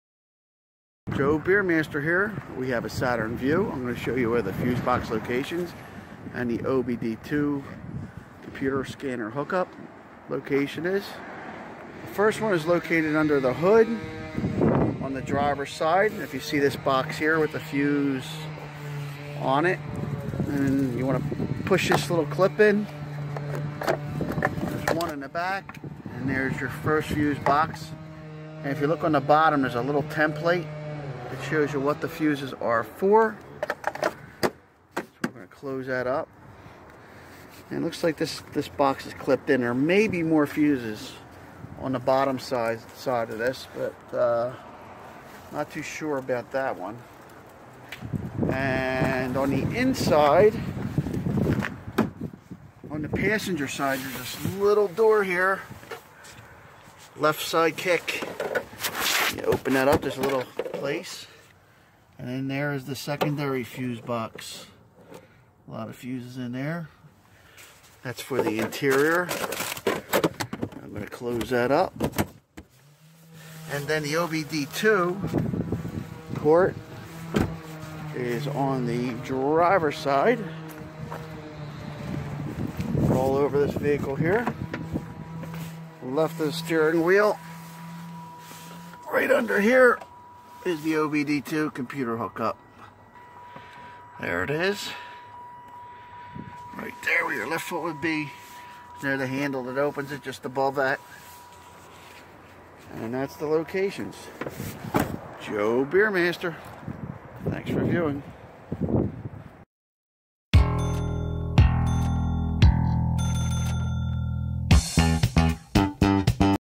Saturn Vue Fuse Box Locations and OBD2 computer Scan Port Location
Saturn Vue  fuse box locations and OBD2 computer scan port hookup locations.